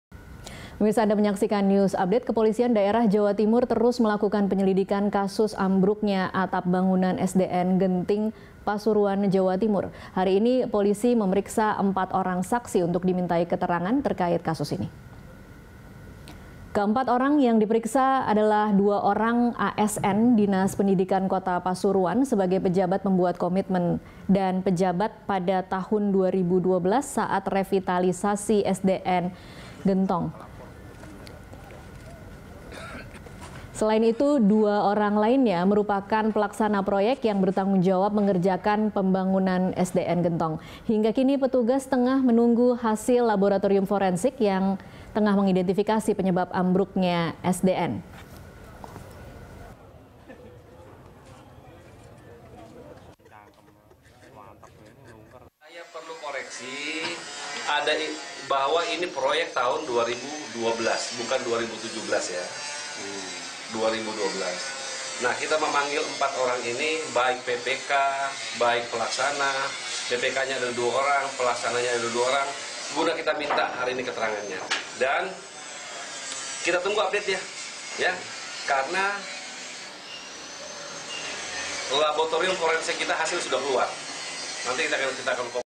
Kasus Atap Sekolah Ambruk, Polda Jatim Periksa 4 Saksi
Aparat Kepolisian Daerah Jawa Timur terus menyelidiki kasus ambruknya atap bangunan SDN Gentong, Pasuruan, Jawa Timur, Kamis (7/11/2019). Polisi memeriksa empat orang saksi untuk dimintai keterangan.

Facebook.com/BeritaSatu
Youtube.com/BeritaSatu